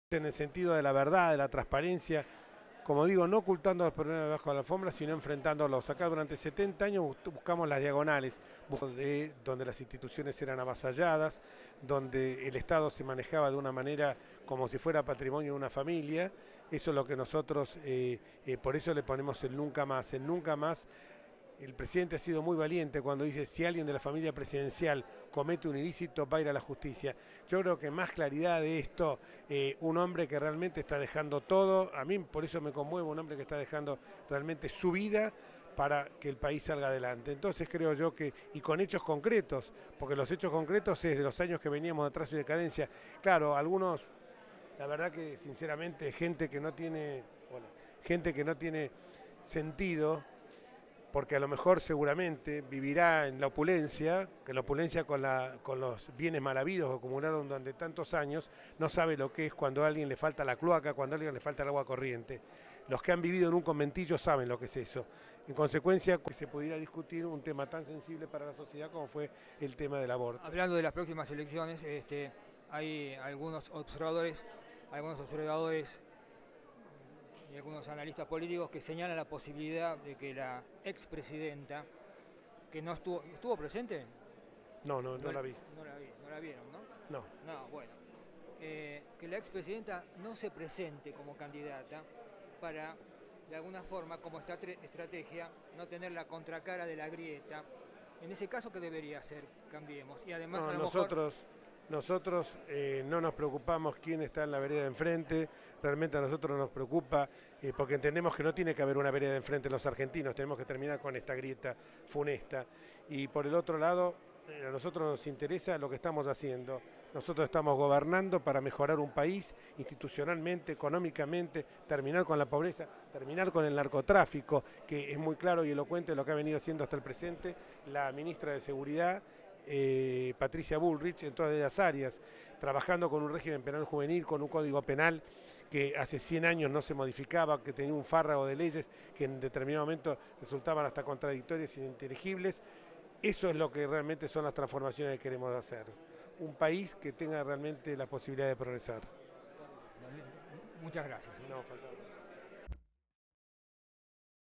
Jorge Ricardo Enriquez, Diputado Nacional
Entrevista realizada por Mario Nacinovich en la apertura de sesiones ordinarias 2019.